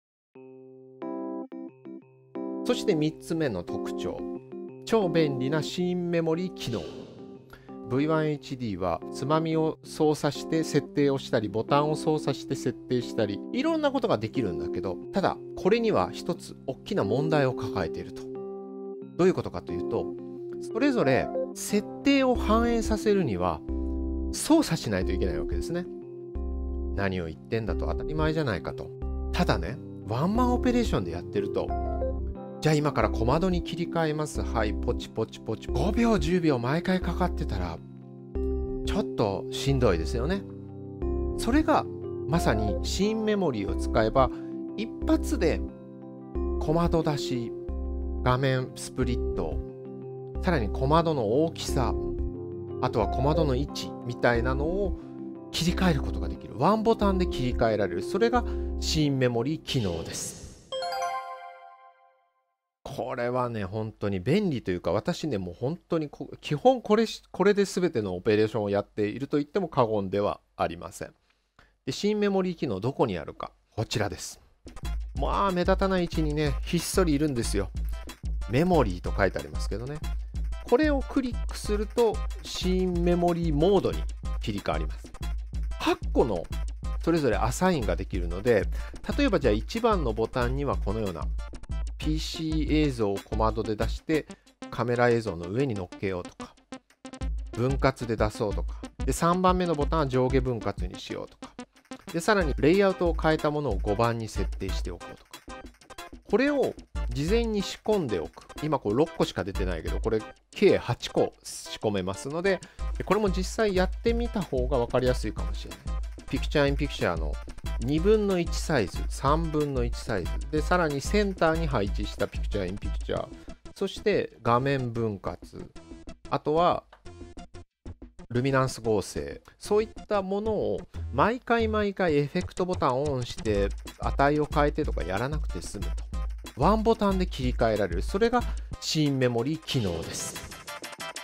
Roland V-1HD☆ワンマンオペレーション使いこなし術③「超便利なシーンメモリー機能」
2020年4月5日、株式会社デスクトップワークスの田口真行が実施したライブ配信セミナー『Roland V-1HD使いこなし術』の中で取り上げた「超便利なシーンメモリー機能」の抜粋ムービーです。

■関連動画

AIが自動で文字起こし＆字幕合成！カット編集もセリフ単位でカンタンにできる動画編集ツール『Vrew』使い方&実演ライブ